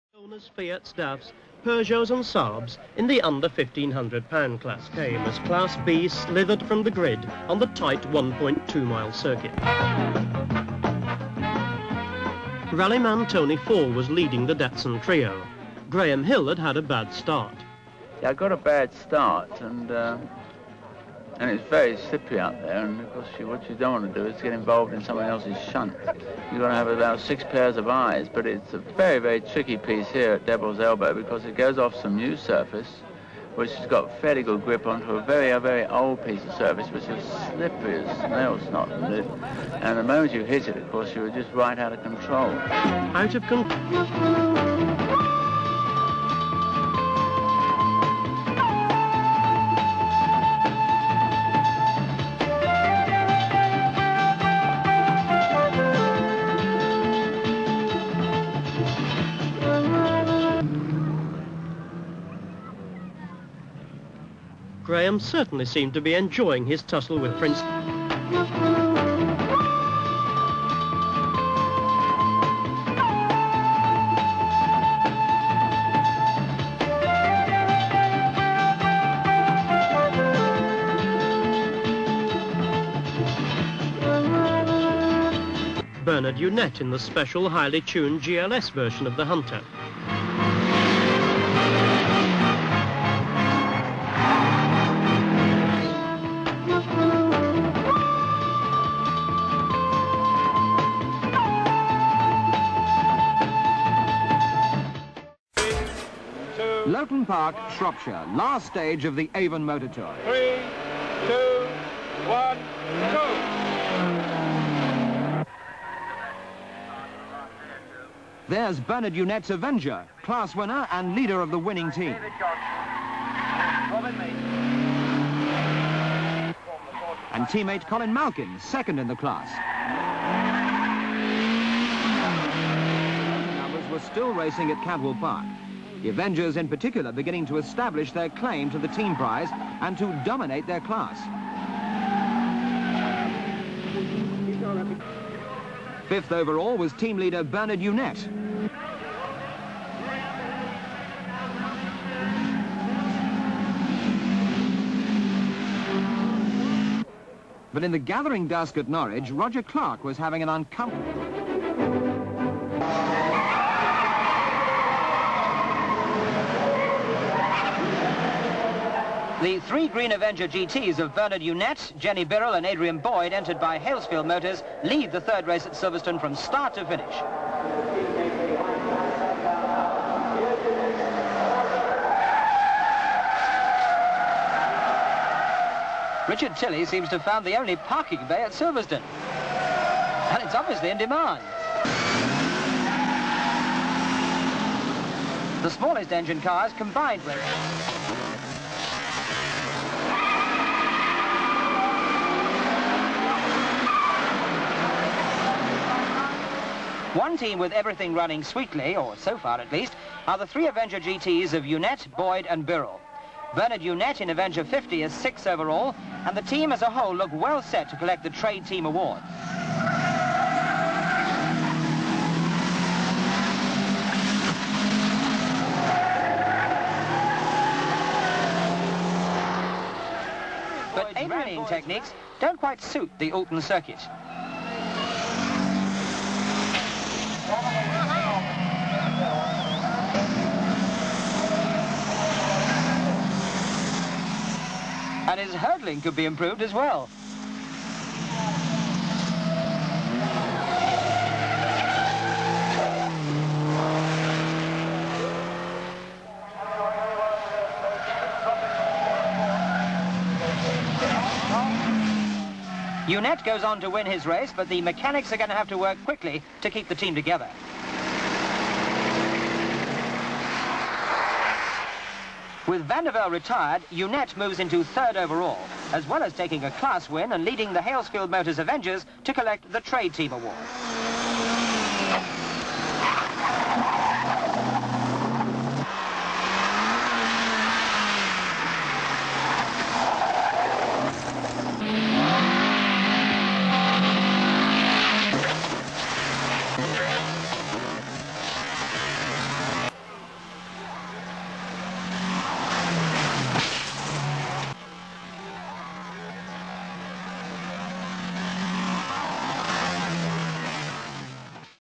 AVENGERS FROM THE PAST
some Hillman Avenger scenes from Avon MOTOR TOUR back to 1973-75